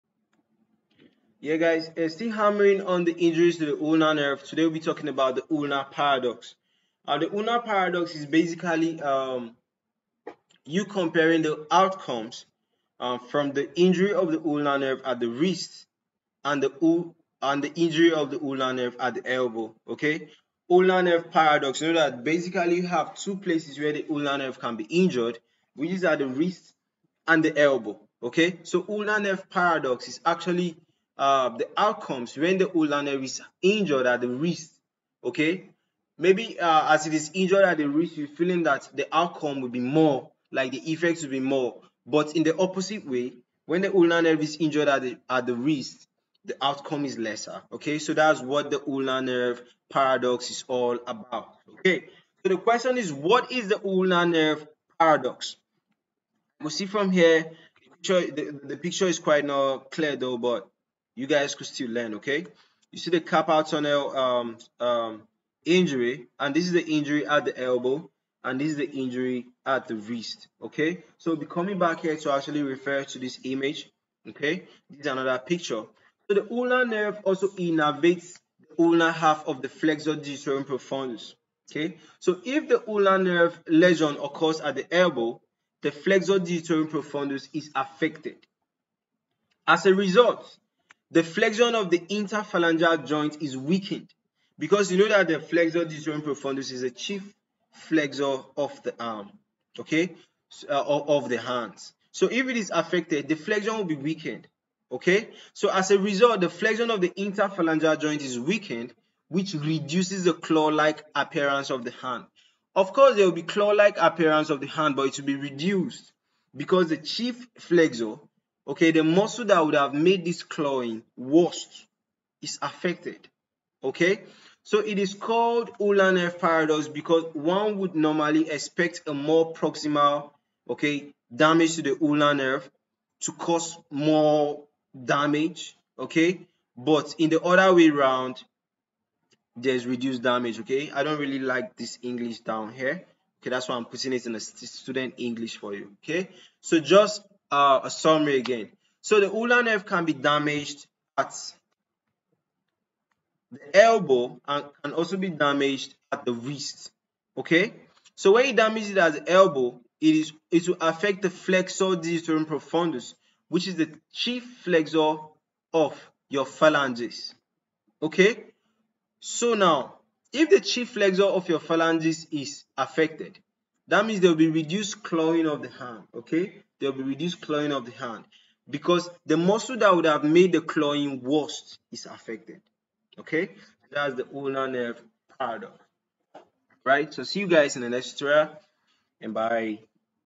Ulnar Paradox; Damage to the Ulnar nerve at elbow affecting flexor digitorum profundus
The ulnar paradox is a phenomenon that occurs when the ulnar nerve is damaged. It is characterized by a less pronounced clawing of the ring and small fingers in more proximal lesions (i.e. around the elbow). The ulnar nerve also innervates the ulnar half of the flexor digitorum profundus muscle (FDP). If the ulnar nerve lesion occurs more proximally, the flexor digitorum profundus muscle may also be denervated .